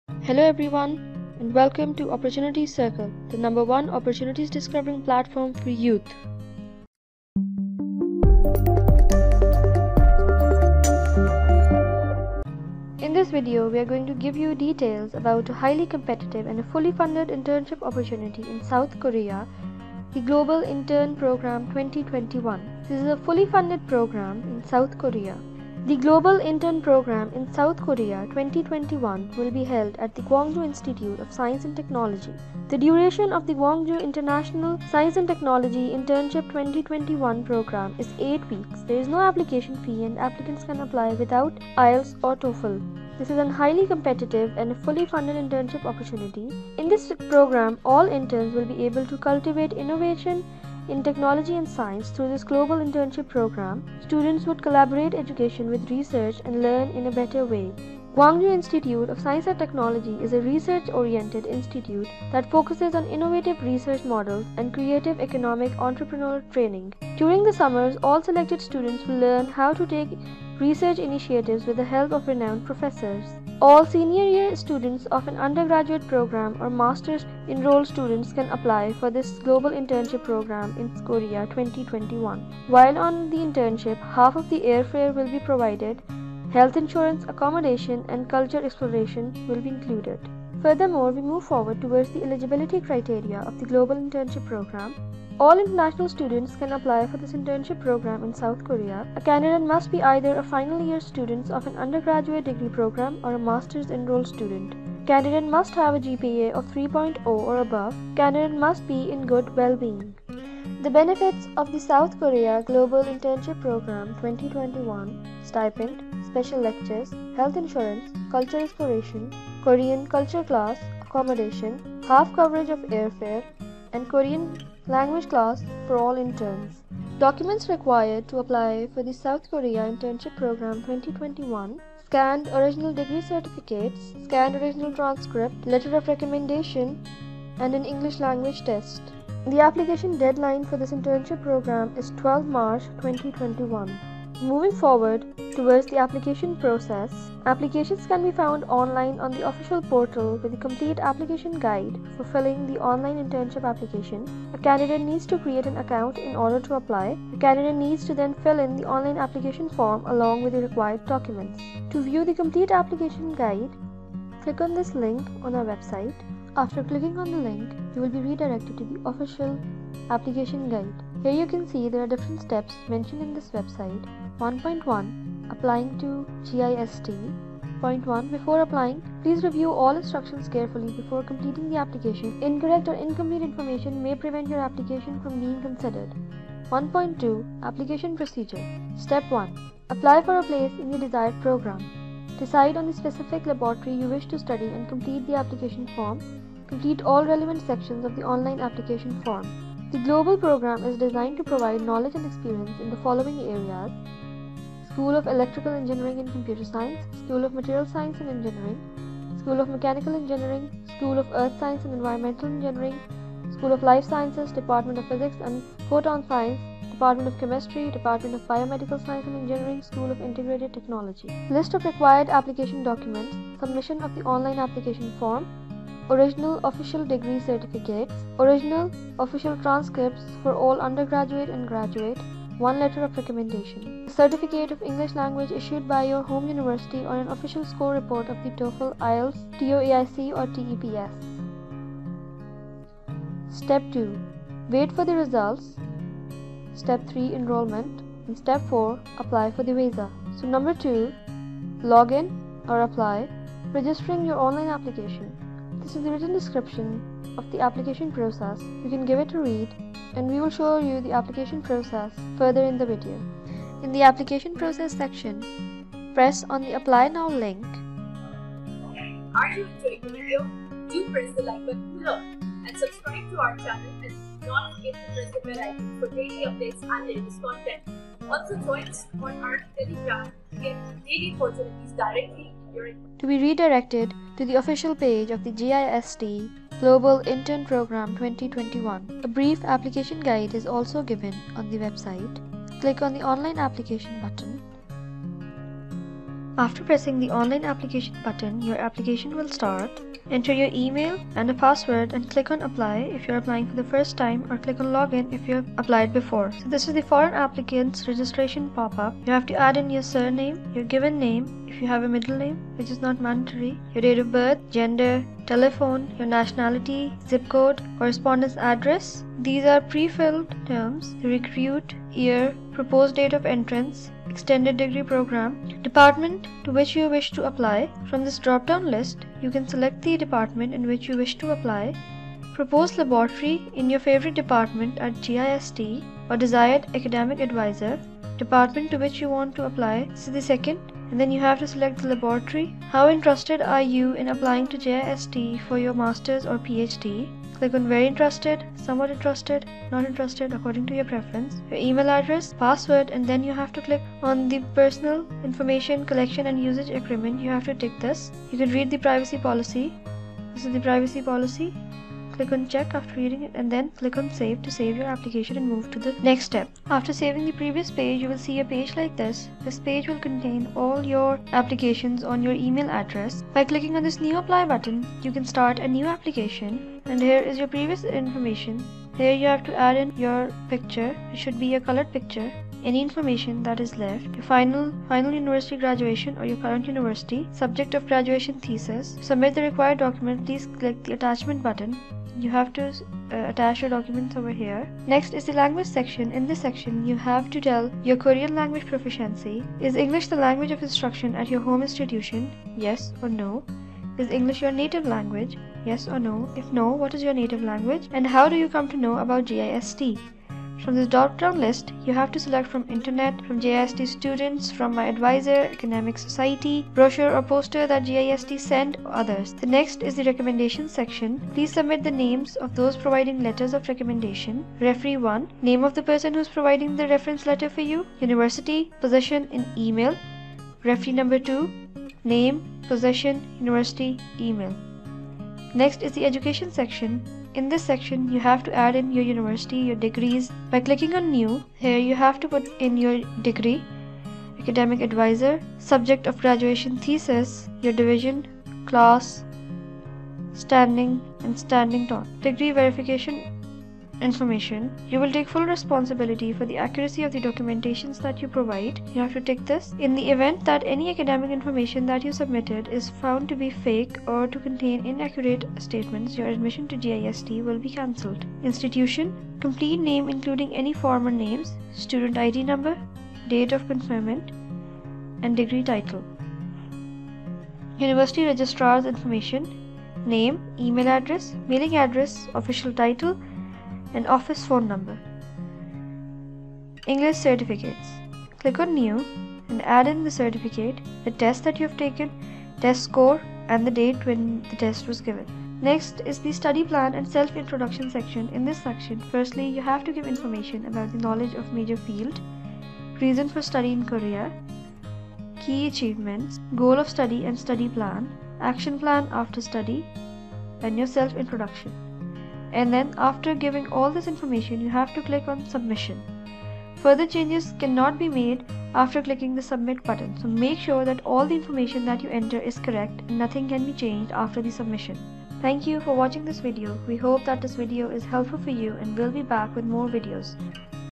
South Korea Internship Program 2023 | Step by step application guide | Fully Funded Internship
Gwangju Institute of Science and Technology South Korea is offering fully funded Global Internship Program 2021 for international Bachelor's, Master's and PHD students who want to explore research environment and opportunities. This internship program is an opportunity to experience how research is conducted on an international level by joining the lab for 8 weeks in the summer.

Make sure you follow our telegram for daily updates inbox